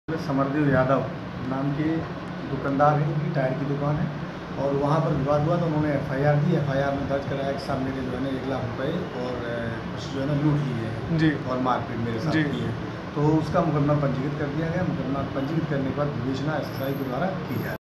बिजनेसमैन से लूट लिए 1 लाख, पुलिस बता रही आपसी विवाद
सुल्तानपुर। जिले में गुरुवार की शाम चांदा कस्बे में टायर व्यवसायी से 1 लाख पांच हजार के लूट का मामला सामने आया है। पुलिस घटना के पीछे दो पक्षों के बीच का आपसी विवाद बता रही है। पीड़ित के तहरीर पर मुकदमा दर्जकर कार्रवाई की जा रही है।